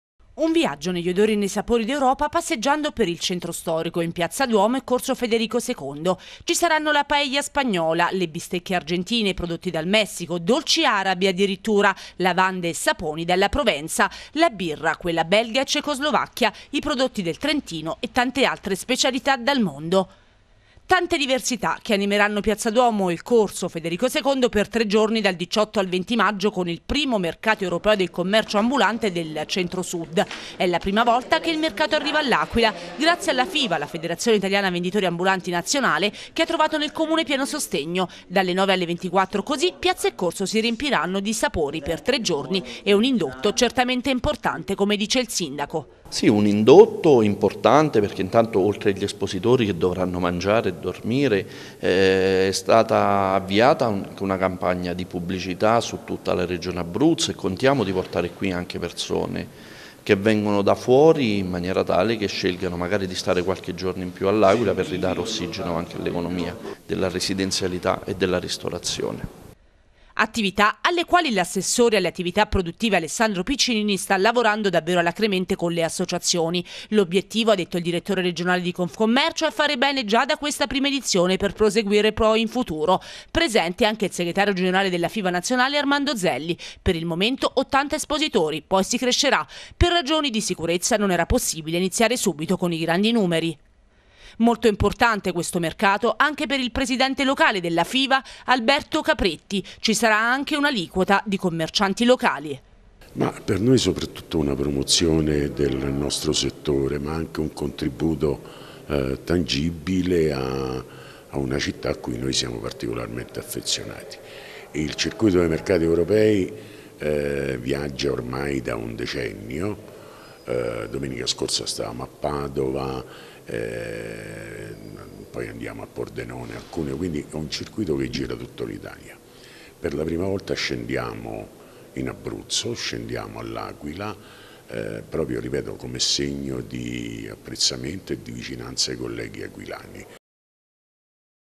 L'Aquila. Mercato europeo del commercio ambulante
Mercato europeo del commercio ambulante. All'Aquila per la prima volta dal 18 al 20 Maggio. Sapori da tutta Europa passeggiando per il centro storico.